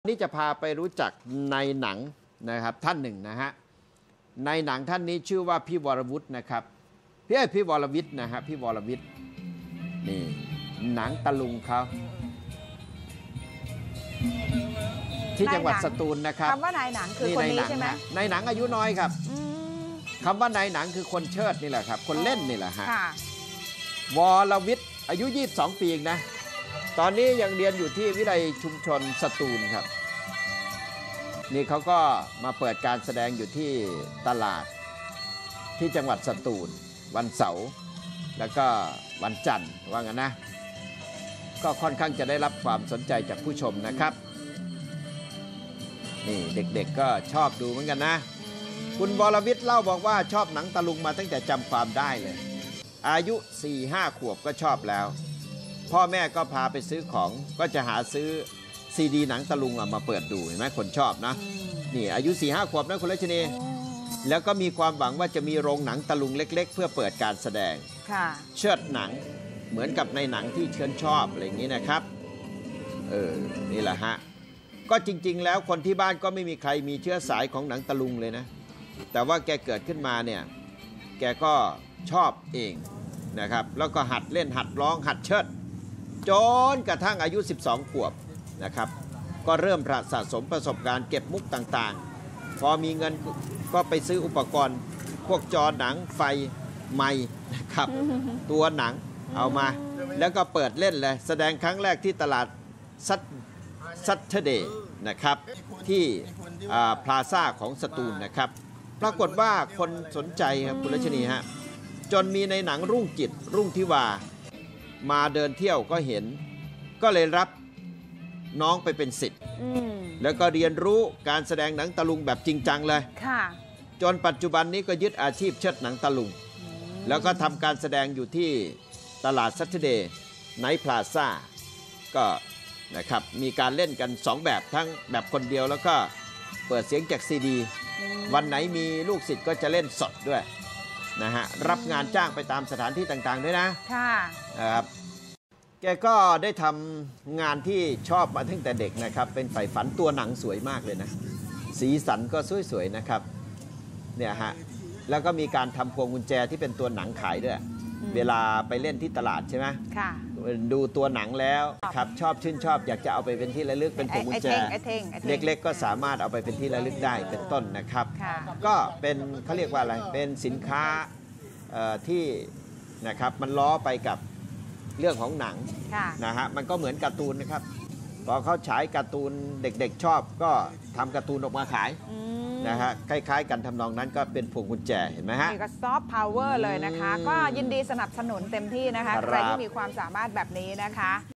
หนุ่มสตูลหลงใหลการแสดงพื้นบ้าน สืบสานศิลปะหนังตะลุง
หนุ่มสตูล หลงใหลหนังตะลุง การแสดงพื้นบ้านมาตั้งแต่เด็ก ฝึกฝนพัฒนาตนเองจนกลายเป็นนายหนัง หารายได้เสริมระหว่างเรียน